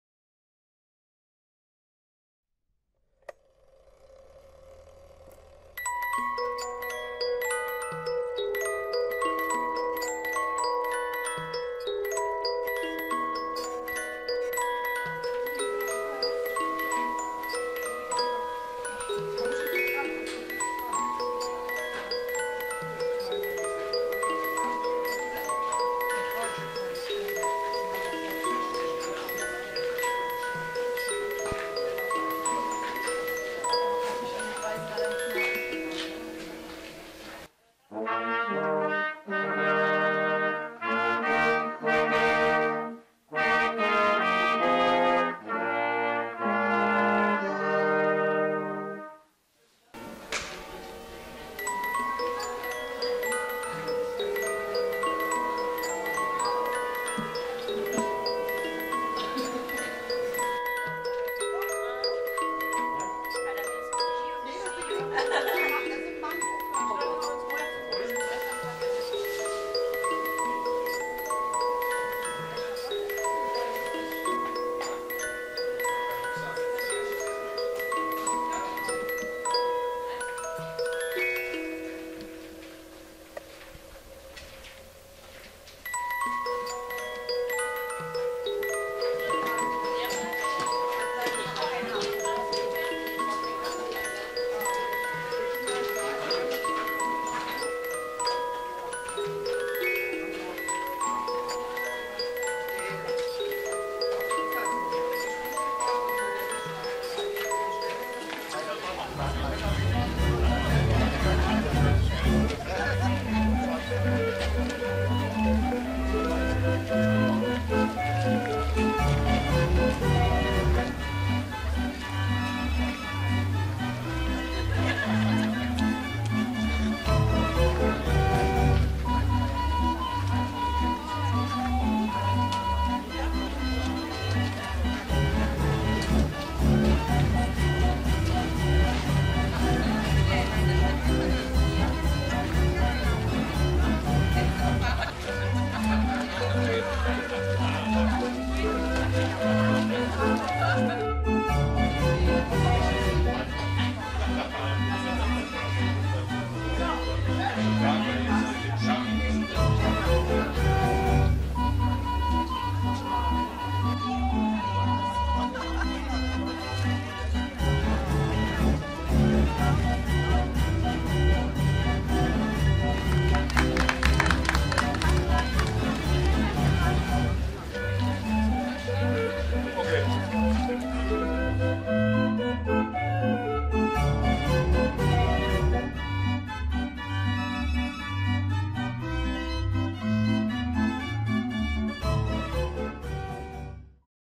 Bundenbach | Weihnachtsmarkt unter Tage
Weihnachtsmarkt im Schieferstollen der Grube Herrenberg in Bundenbach.